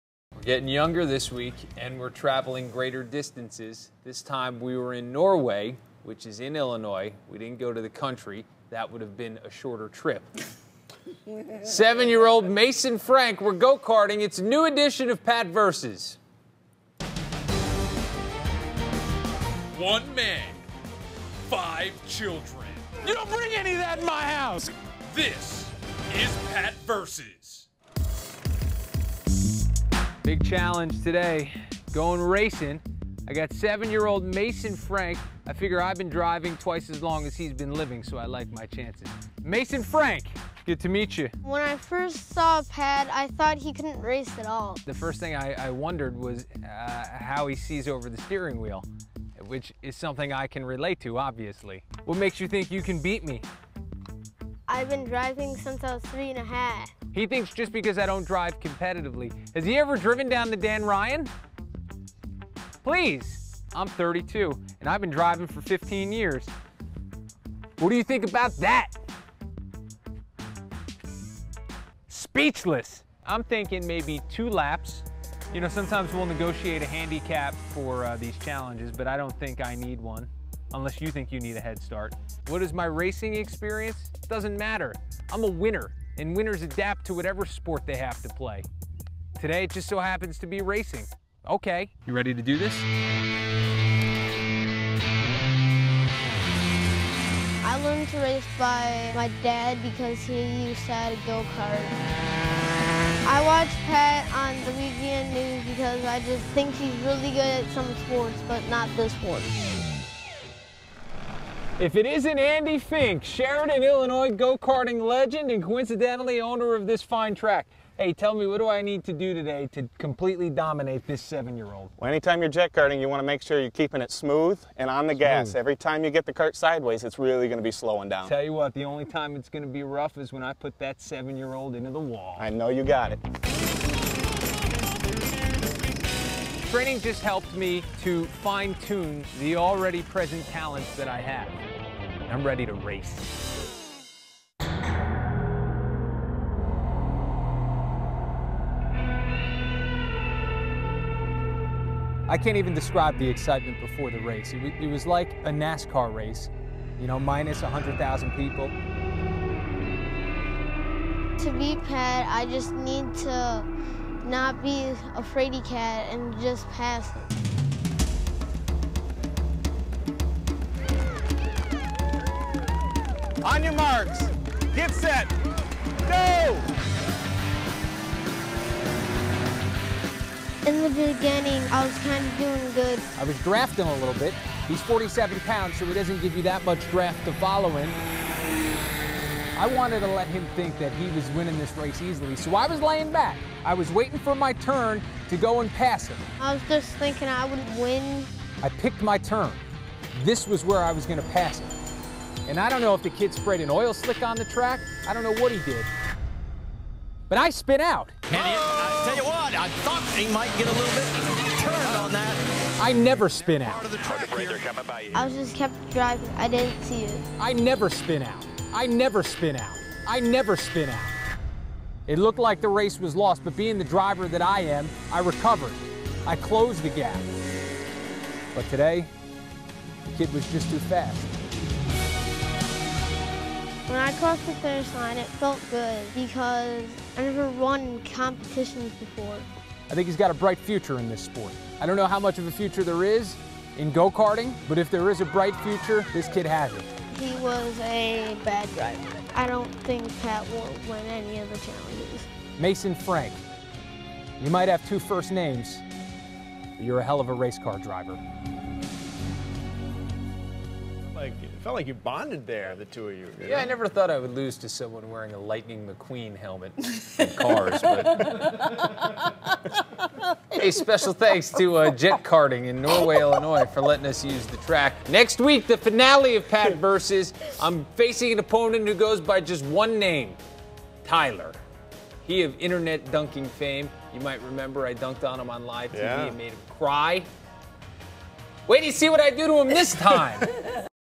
Pat Vs- Go Karting
The FULL EPISODE. Grown man races 7-year old. Enjoy.